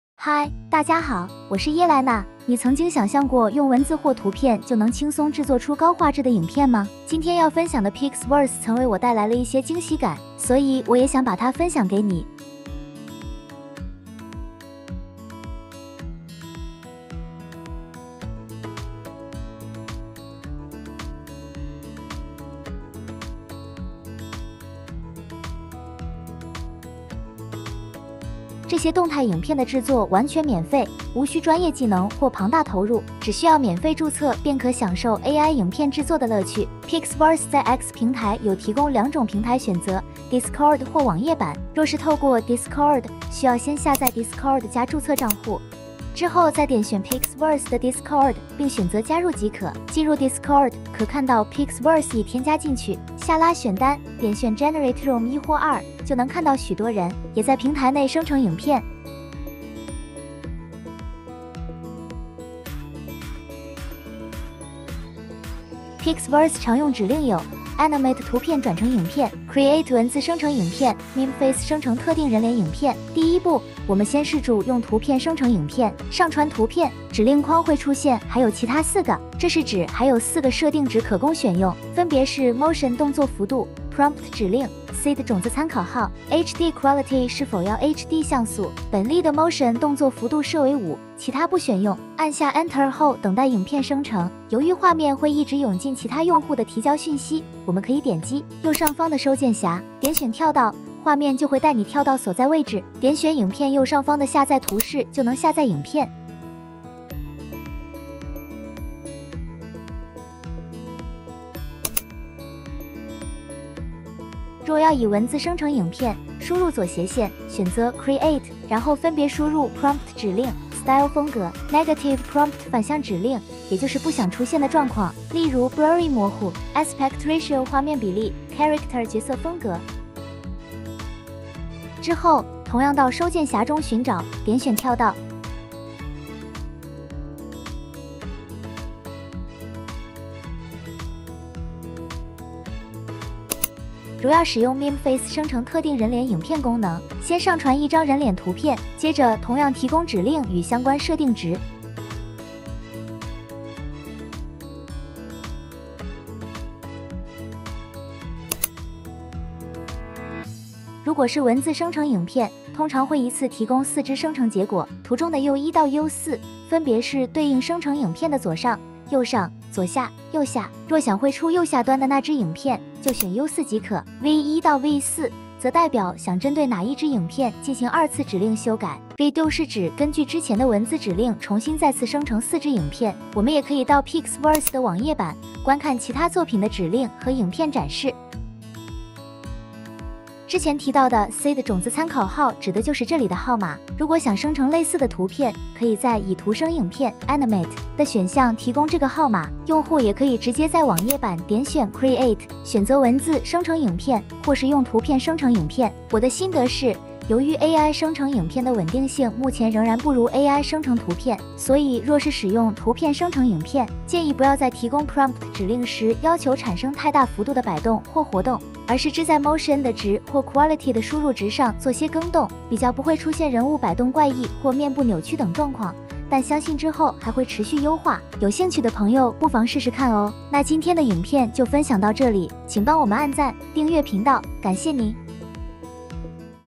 【免費 AI 影片製作】PixVerse 文字生成影片+ 圖片生成影片，完全免費，Discord 或網頁版都可用，讓你的創作更豐富多元！Free AI video Generator
免費 AI 影片製作工具 PixVerse 提供文字生成影片、 圖片生成影片、替換人臉/仿製人臉影片 (meme face)，用戶可選擇使用 PixVers 的 Discord 平台或PixVers 網頁版，豐富你的創作能力! (pika ai 替換選項)

分享各種自媒體 、AI 、生產力工具、斜槓課 (付費&免費) 的真實經驗+超值乾貨知識分享

00:00  用文字和圖片生成高畫質影片 PixVerse
00:11  高畫質影片範例展示
00:36  PixVerse 提供的操作平台
00:49  PixVerse 在 Discord 平台的使用方式
01:13  用圖片生成影片
01:54  用文字生成影片
02:23  生成特定人臉影片/換臉影片
02:40  補充說明
03:04  PixVerse 網頁版
03:25 使用心得